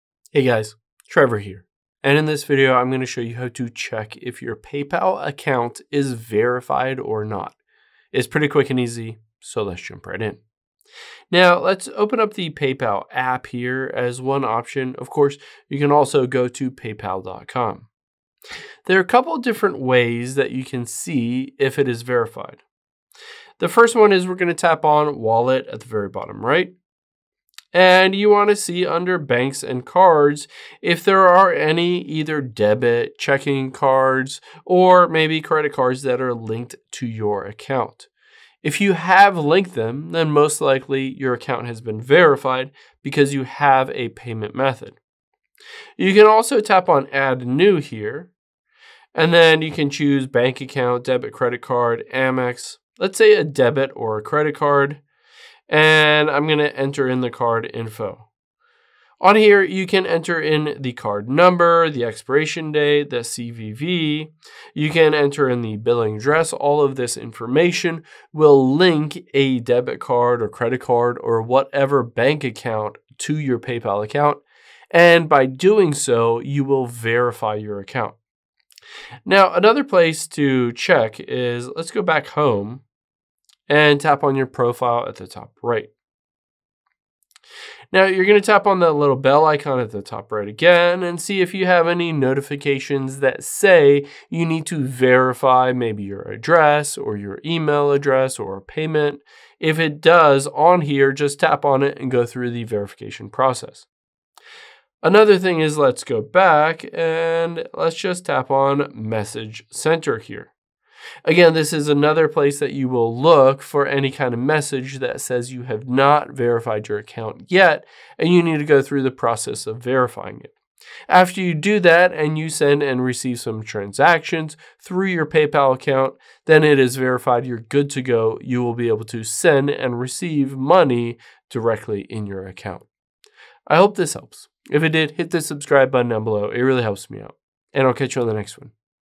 How To Check If PayPal Account Is Verified Or Not
Here's how to find out if your PayPal account is verified for the bank account, email, address, etc. and how to verify it if not.